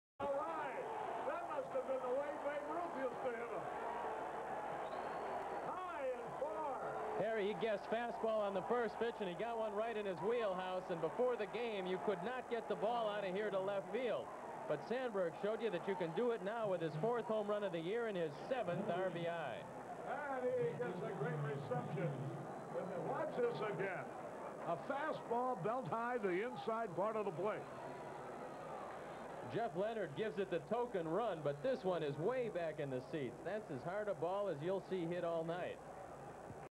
Replay of Sandberg HR in SF 5/8/85
Sandberg hits a long home run against Mike Krukow.